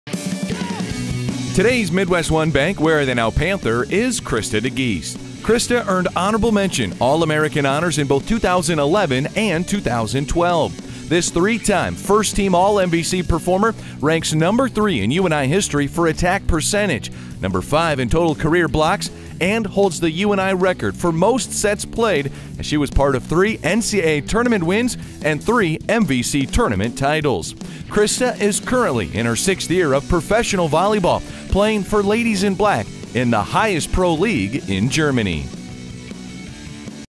UNI Panthers Where are they, Krista DeGeest - UNI Volleyball
Krista earned Honorable Mention All-American honors in both 2011 and 2012. The three time first team all-MVC performer ranks  3 in UNI history for attack percentage, number 5 in total career blocks, and holds the UNI record for most sets played as she was a part of 3 NCAA Tournament wins and 3 MVC Tournament titles. Krista is currently in her 6th year of professional volleyball playing for Ladies in Black in the highest pro league in Germany.